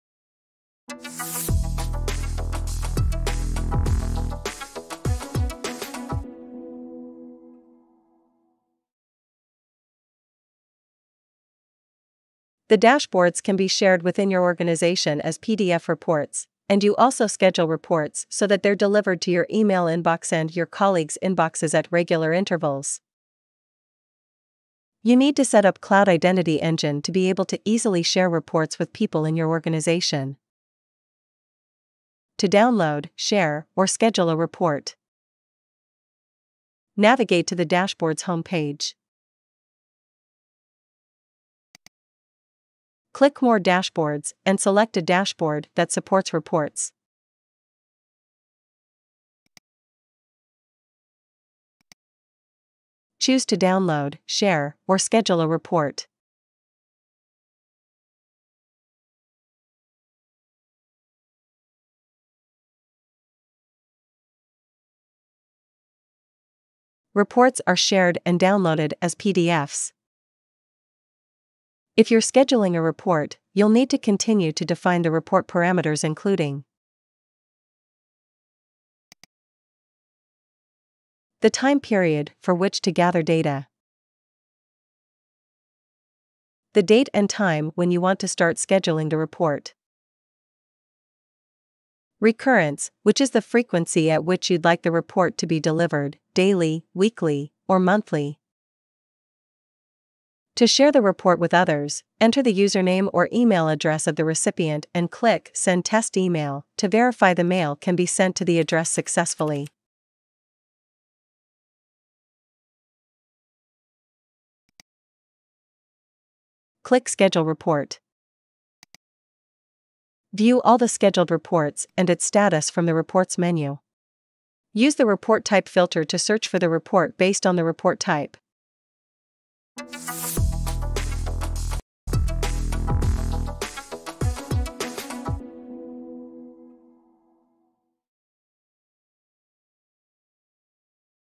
Schedule reports in Strata Cloud Manager
This video shows you how to schedule reports in Strata Cloud Manager